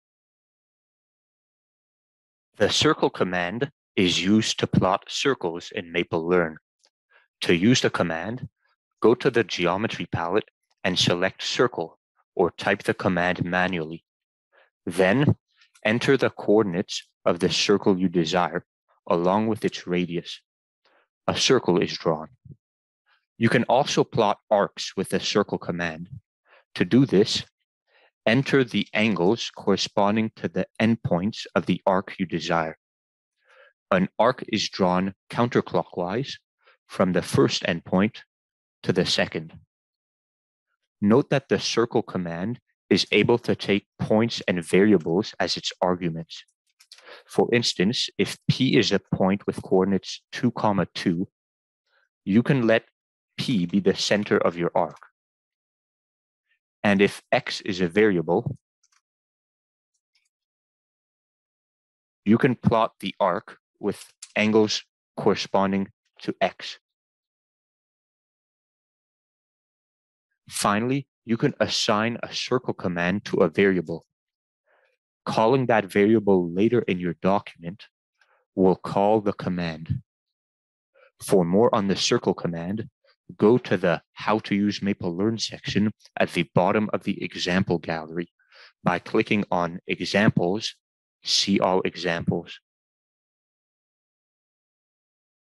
Maple Learn: The Circle Command
In this video, we show you how to use the circle command in order to plot circles on the graph in your documents.